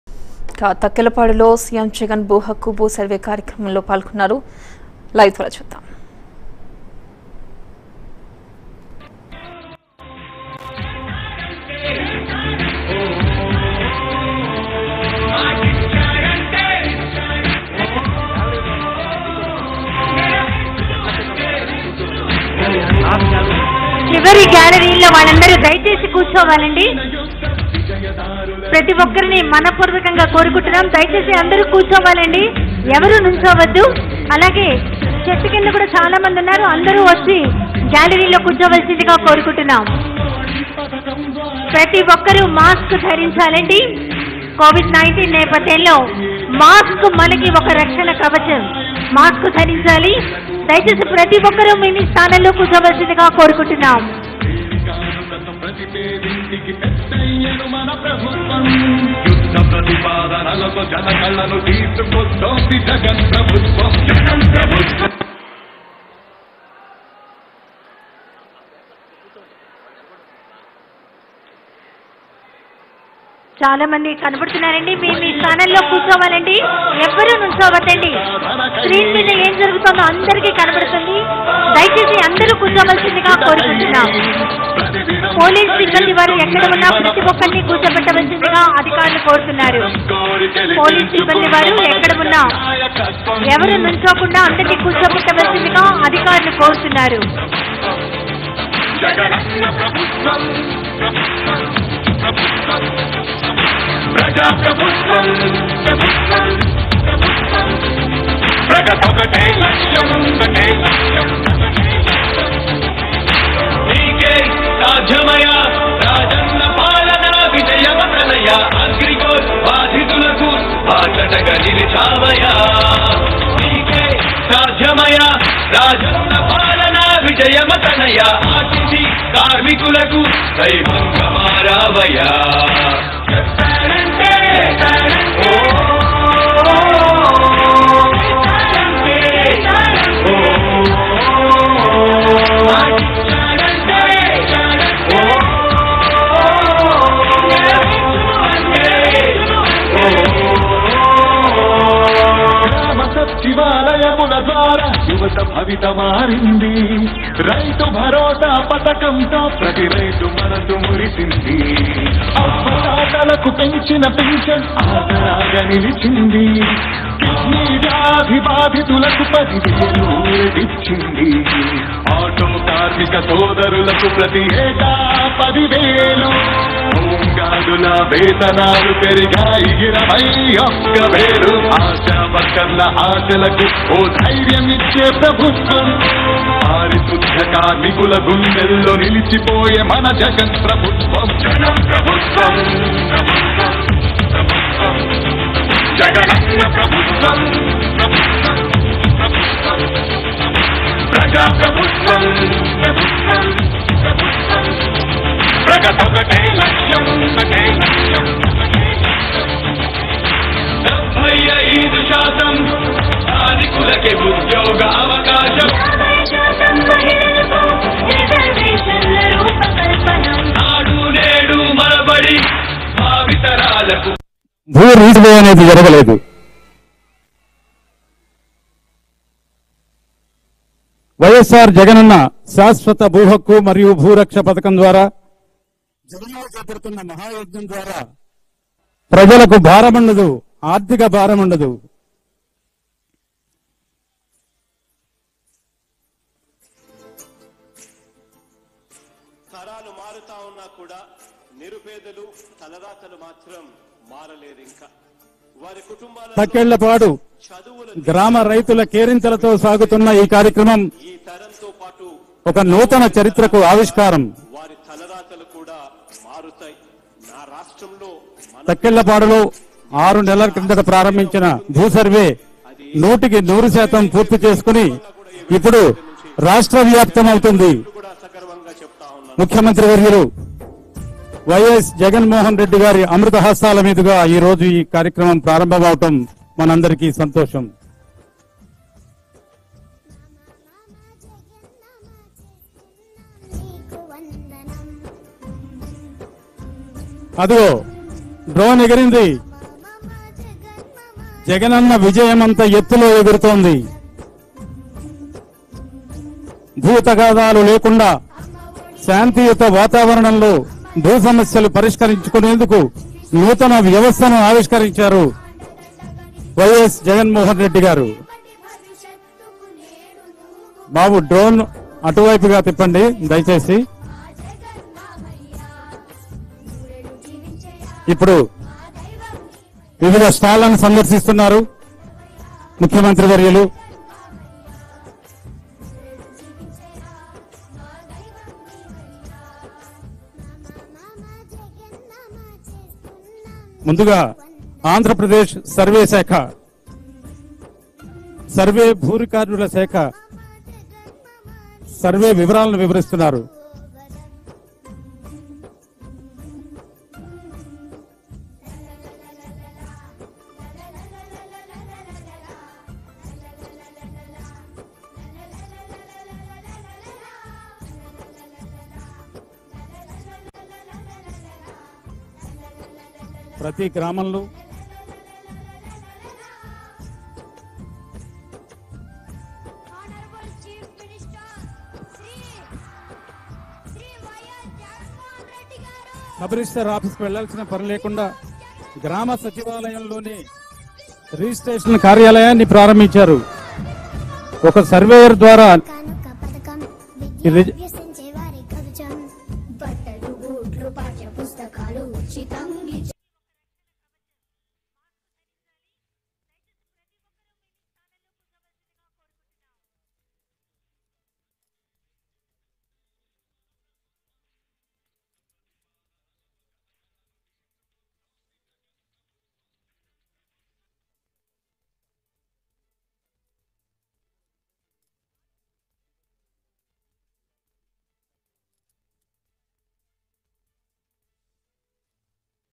AP CM YS Jagan Grand Entry At SGN College In Jaggayyapeta | Jagananna Bhoo Hakku Bhoo Raksha Scheme
AP CM YS Jagan Grand Entry At SGN College In Jaggiahpet | Jagananna Bhoo Hakku Bhoo Raksha Scheme